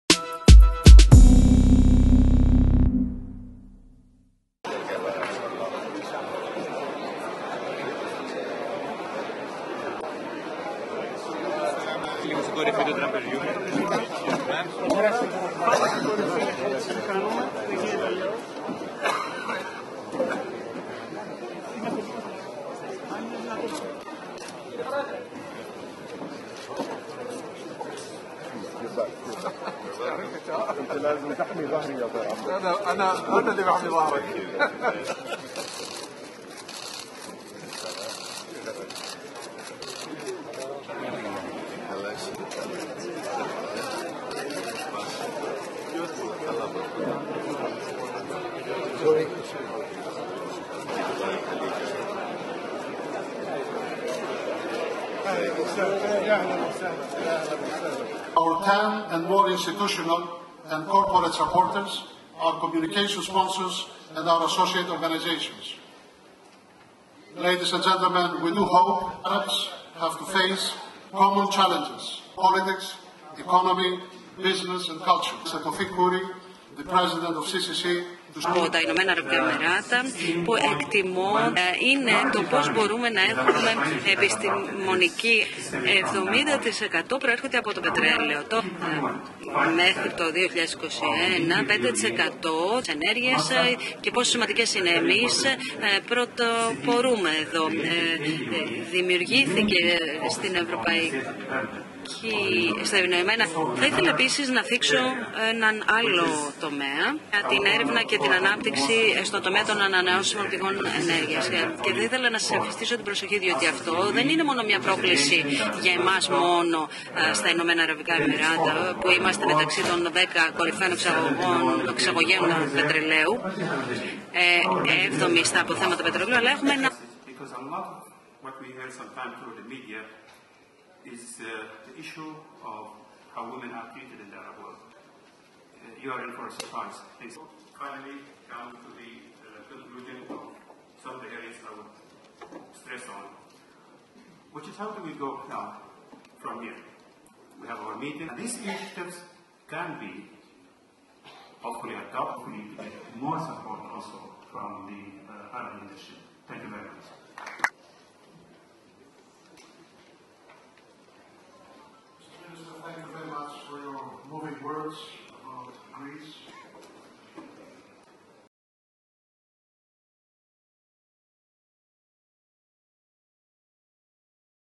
Δεύτερη Ευρωαραβική Διάσκεψη υπό την επωνυμία «Προς μια Σταθερή Συμμαχία»
Εσωτερικά πλάνα από την δεύτερη Ευρωαραβική Διάσκεψη υπό την επωνυμία «Προς μια Σταθερή Συμμαχία».ΑΠΕ-ΜΠΕ/Α.ΡΕΜΟΥΝΔΟΣ